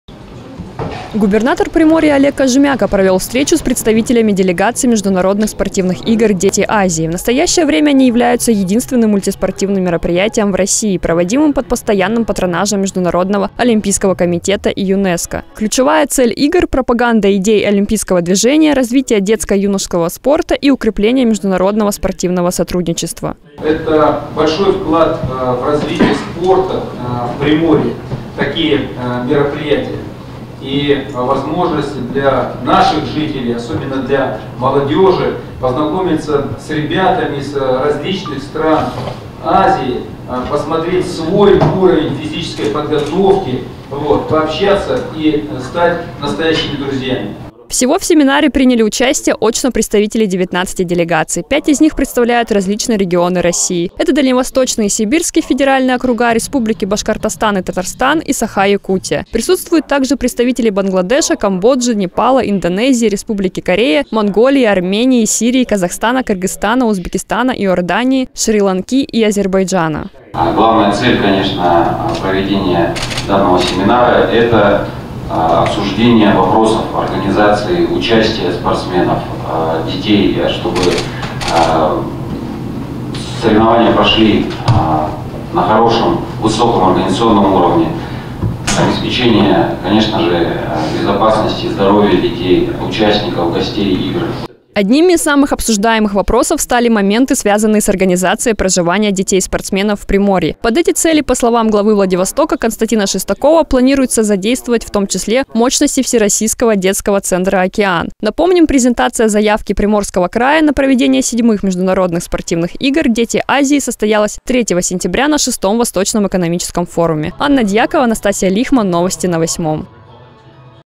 Дети Азии
Губернатор Приморья Олег Кожемяко провел встречу с представителями делегаций международных спортивных игр «Дети Азии».